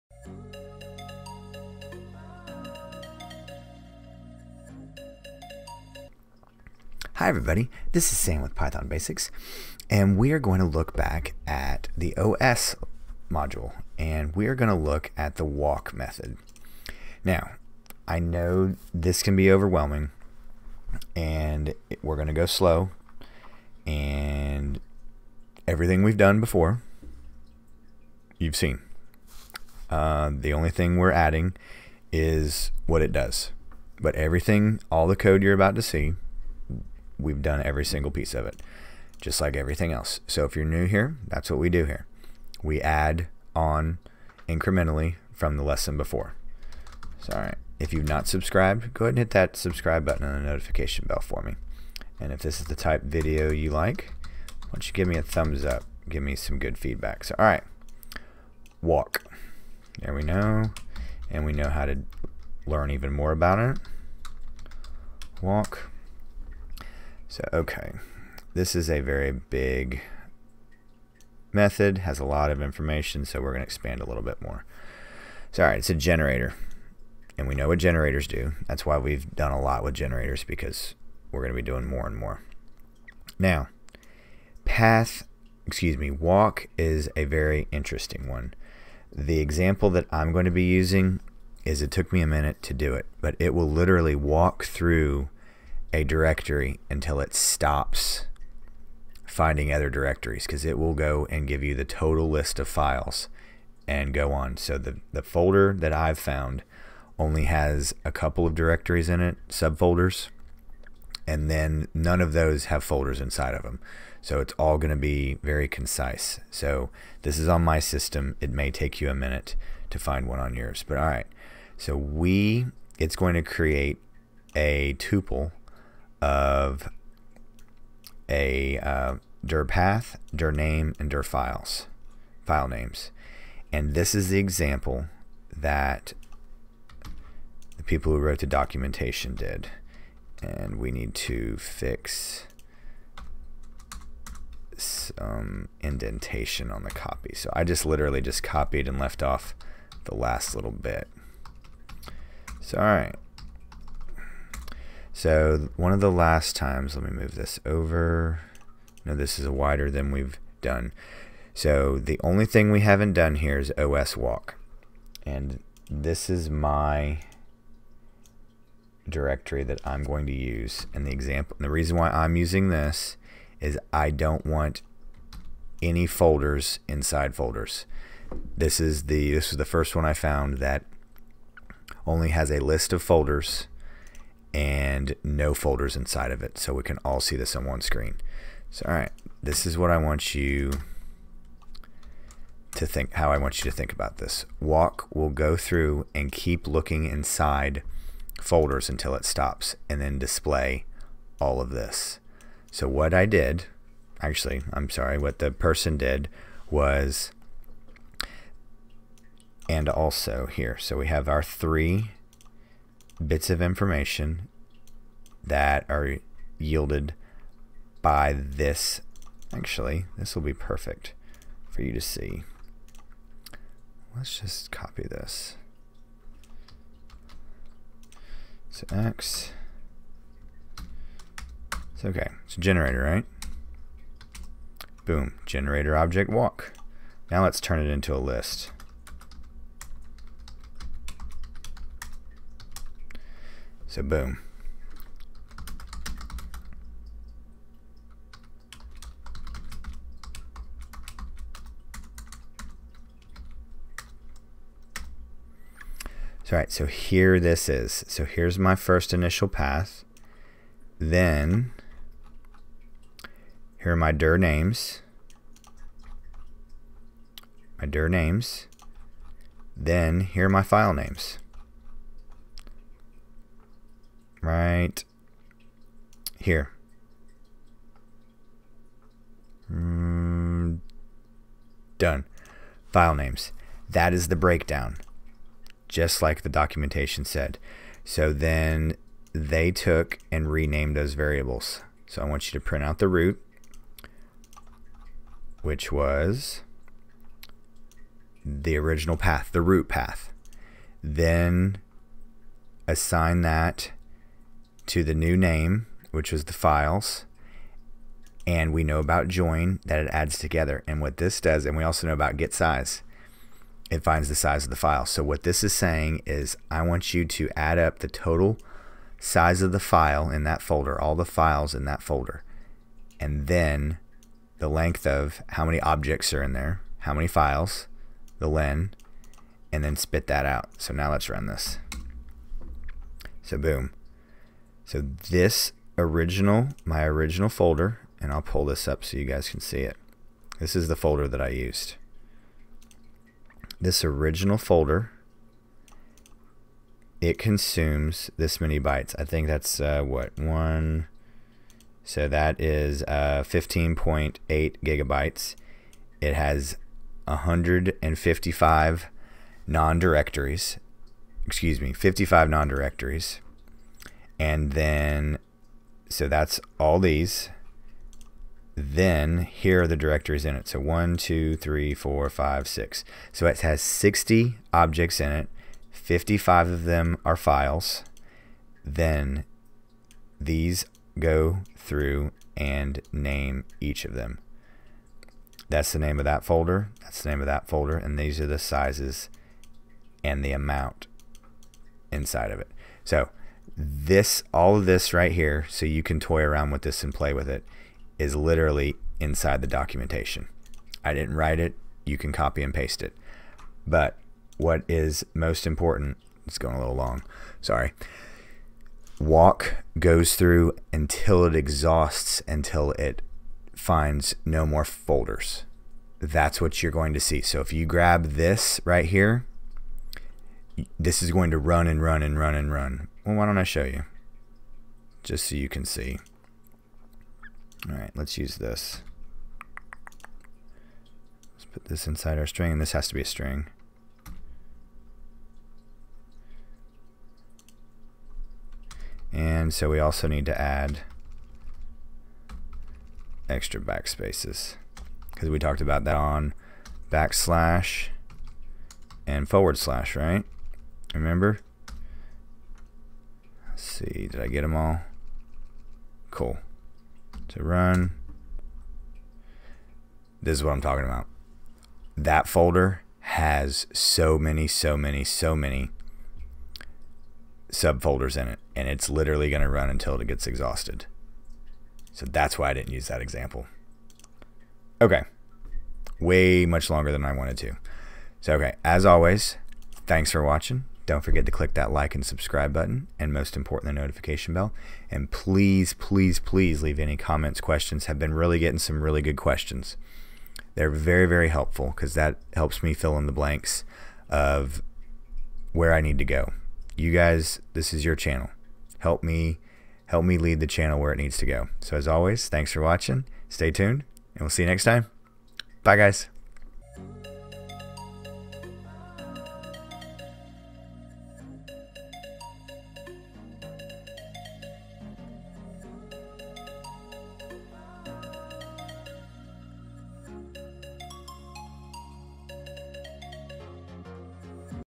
Python Basics OS Walk Method
Learn how to use the walk method from the os module for python programming.  The walk method works through folders until each folder only has files remaining.